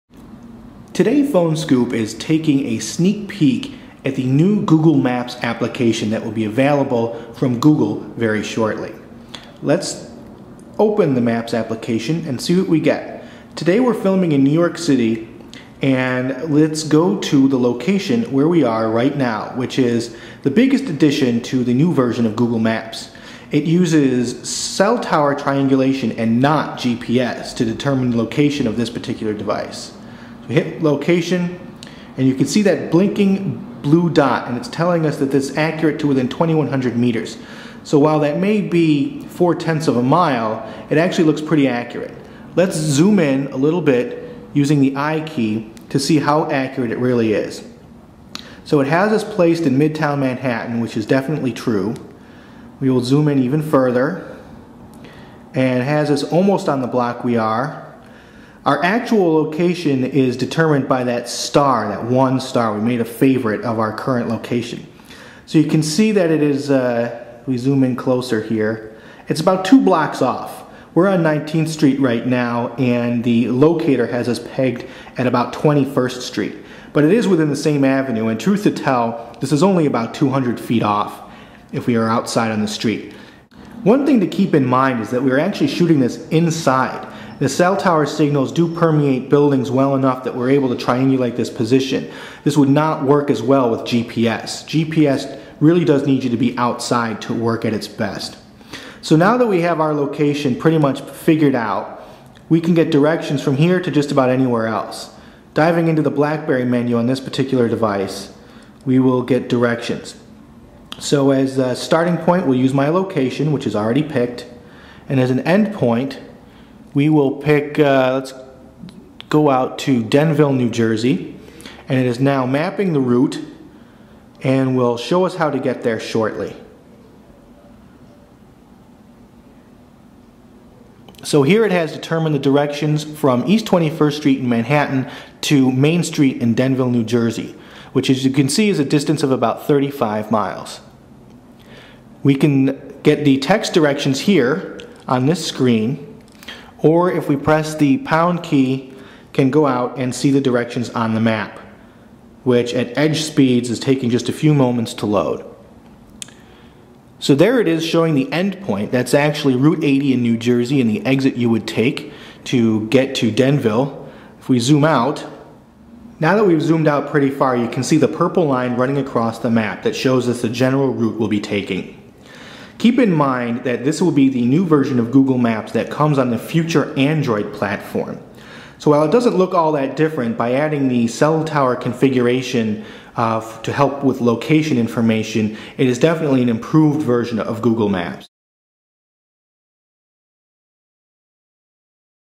Google Maps
This is a demonstration of the new Google Maps application. It uses cellphone tower triangulation to help uses find their location and directions.